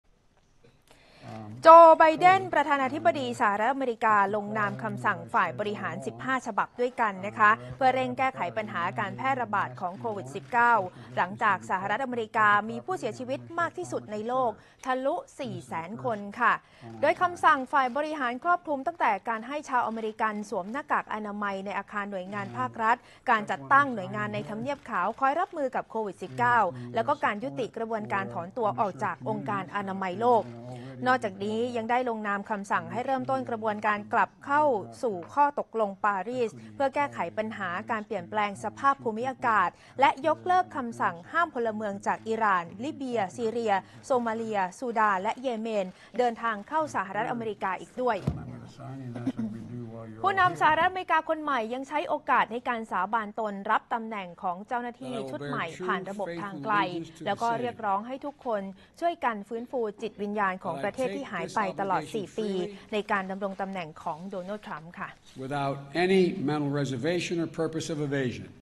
"ไบเดน" ลงนามคำสั่งรับมือโควิด-19 - รื้อนโยบาย "ทรัมป์" (21 ม.ค. 64)
ผู้นำสหรัฐอเมริกาคนใหม่ เรียกร้องให้เจ้าหน้าที่ช่วยกันฟื้นฟูจิตวิญญาณของประเทศให้กลับมาอีกครั้ง นอกจากนี้ยังลงนามคำสั่งฝ่ายบริหารเร่งควบคุมการแพร่ระบาดของโควิด-19 และพลิกนโยบายของ โดนัลด์ ทรัมป์ ตลอด 4 ปีที่ผ่านมา